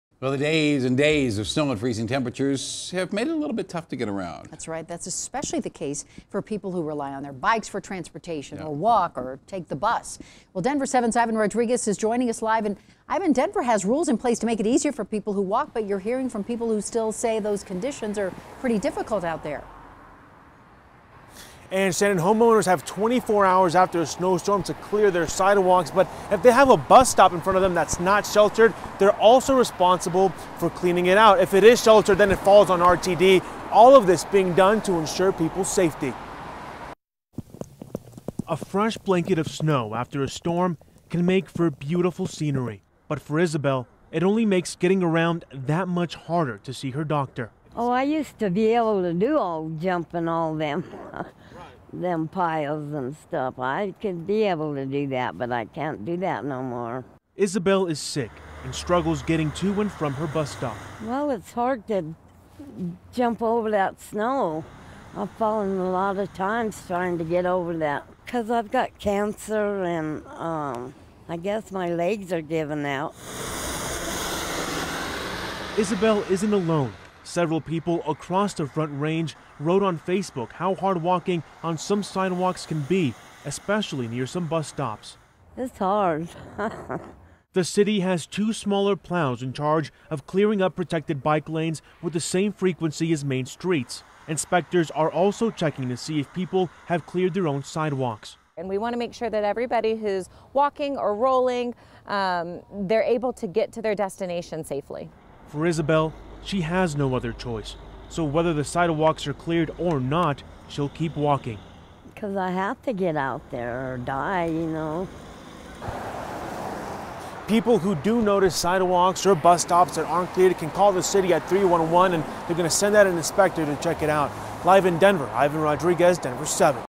Denver says it has different ways of making sure everyone has a safe commute after a snowstorm
The commute can become more difficult for Denverites following a snowstorm. This is especially true for people who ride their bikes or have to walk on sidewalks and use the bus. But the city says it has ways to make sure the commute isn’t as tough.